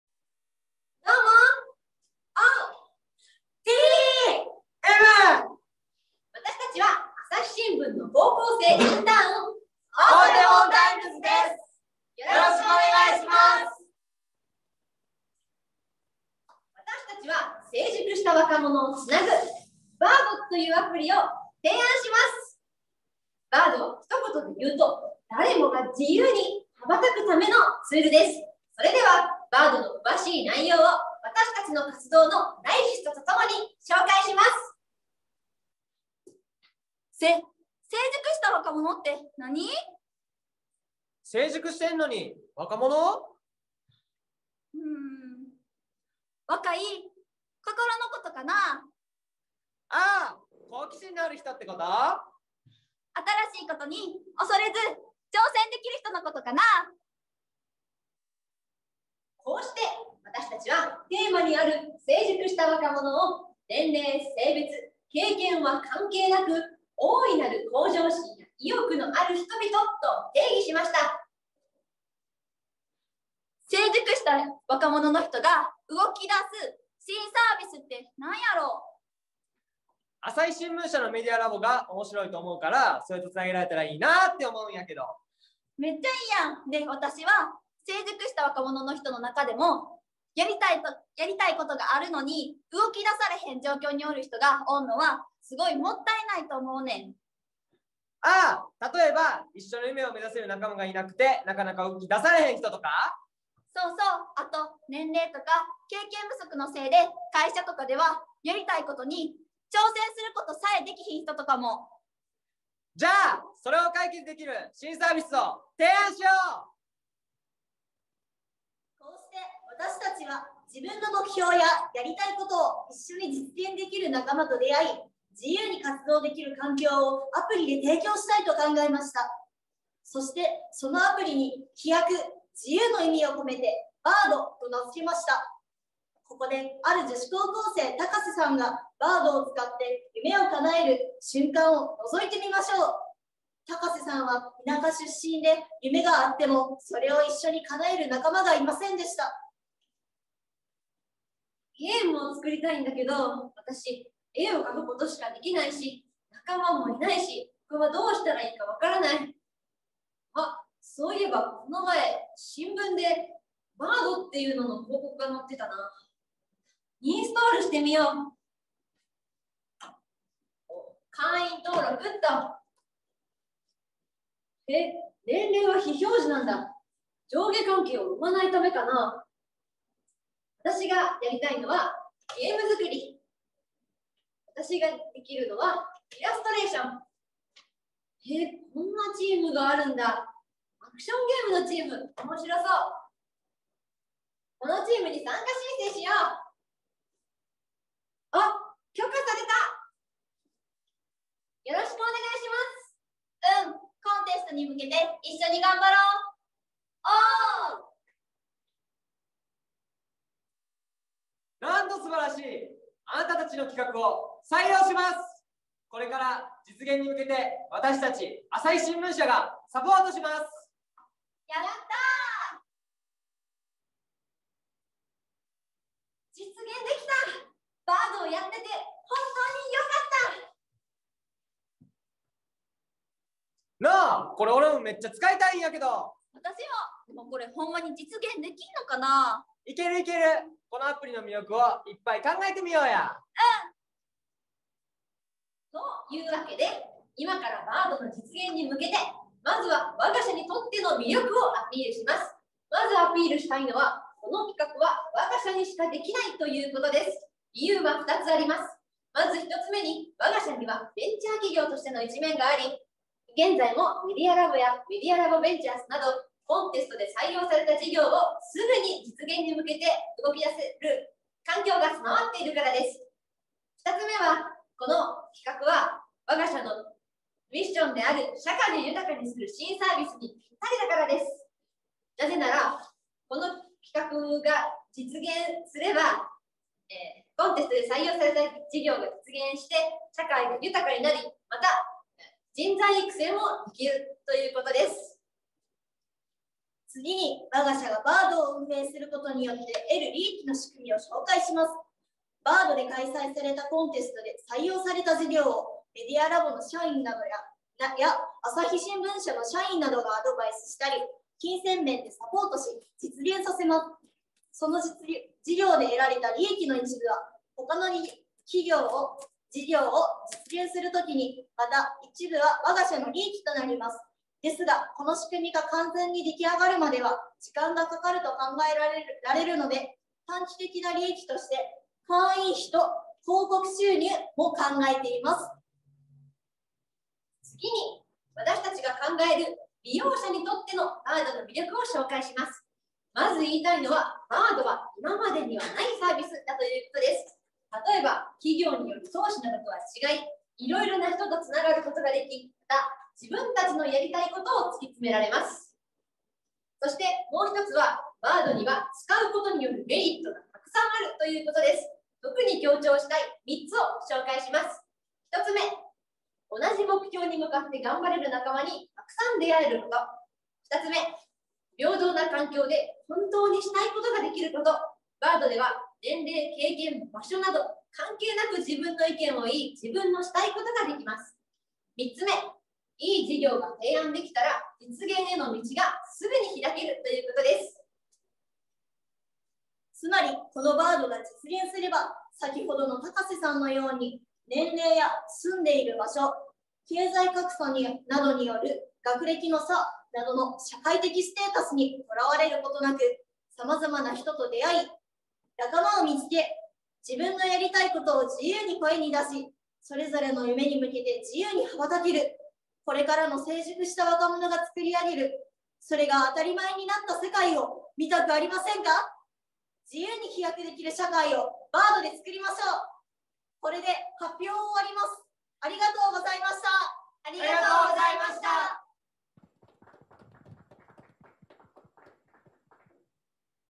クエストカップ 2021 「コーポレートアクセス」部門 朝日新聞社賞 追手門学院大手前高等学校 チーム「Bird」
中高生の探究学習の祭典、「クエストカップ 2021 全国大会」！

実在する企業のミッションに取り組み、自分たちならではの視点から探求した答えをプレゼンテーションするコーポレートアクセス部門に出場し、

朝日新聞社の企業賞を獲得した
追手門学院大手前高等学校「Bird」の発表です。

グランプリ受賞チームのインタビューも

◆クエストカップとは

クエストカップ全国大会は、 生徒、先生、企業人1,000名以上が一堂に集まる日本最大規模のアクティブ・ラーニング型の学習発表会 。探究学習プログラム「クエストエデュケーション」に取り組んだ小中高生が、自らの探求の成果を社会に向けて発信する場です。 2006年から毎年開催され、2021年で16回目を迎えました。

◆コーポレートアクセスコースとは

実在する企業へのインターンシップを教室で体験し、働くことの意義や経済活動について学びます。企業という仕組みを活用して、自分たちの手で未来をつくることを学ぶ探究型のプログラムです。
前半はフィールドワークやアンケート調査など企業の実務を体験し、後半では、起業から出される課題(ミッション)に取り組みます。企業理念を踏まえたリアルな課題に取り組むことで、社会や企業に対する興味・関心が生まれ、生徒一人ひとりの主体性と創造性を育むと共に、職業観を醸成します。